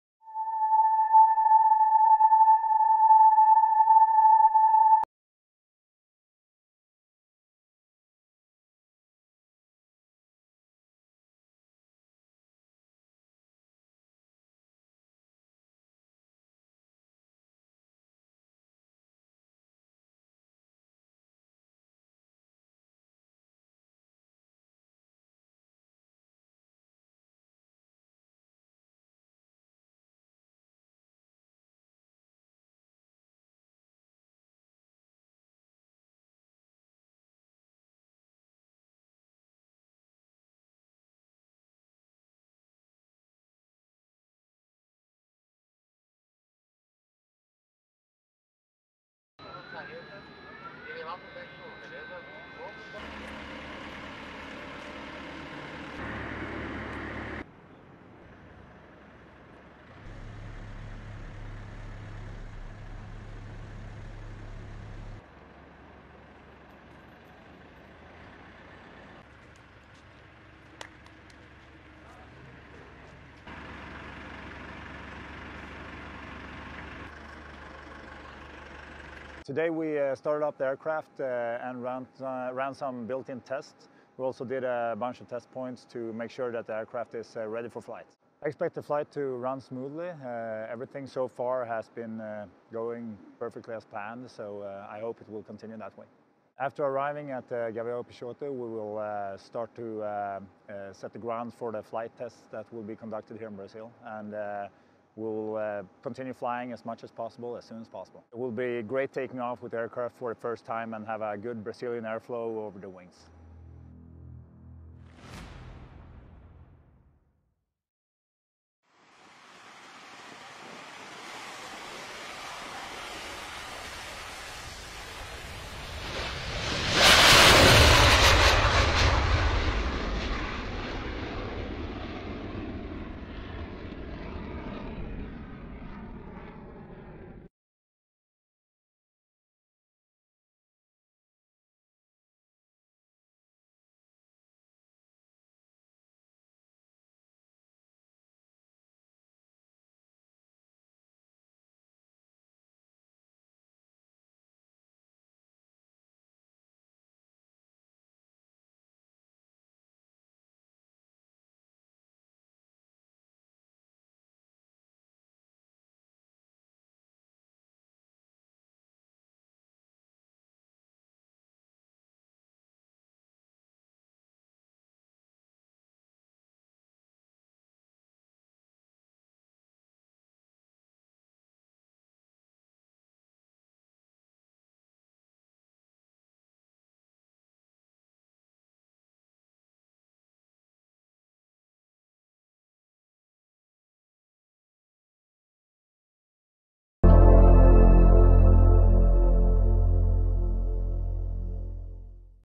SAAB Gripen E in Brazil - arrival, from port to hangar, ground testing and first flight
[video: SAAB AB / Força Aérea Brasileira]